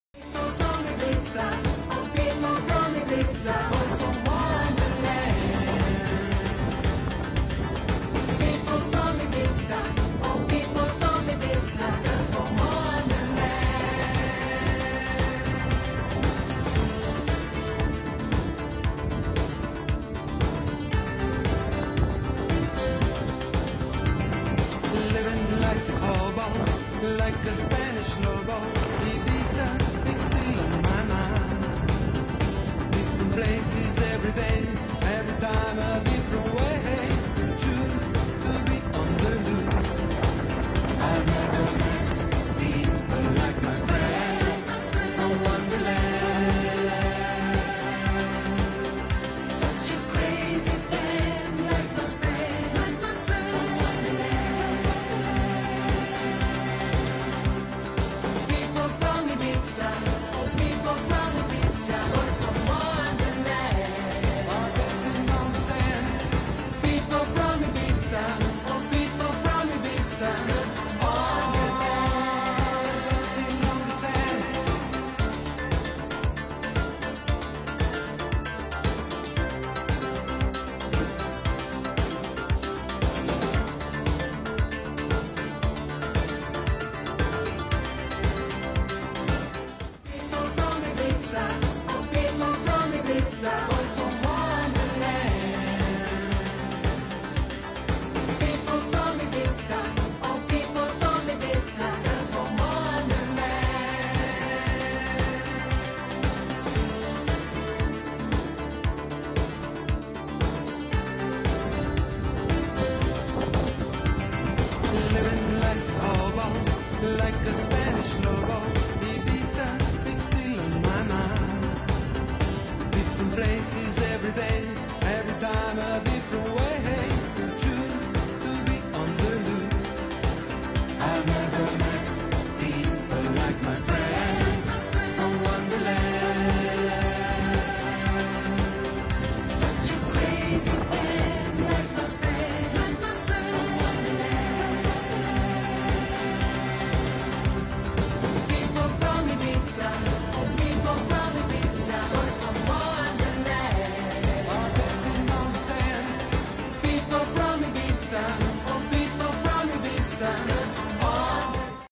Ibiza , castillo
recordando mi infancia en Ibiza en los 70s y 80s cuando vivía aquí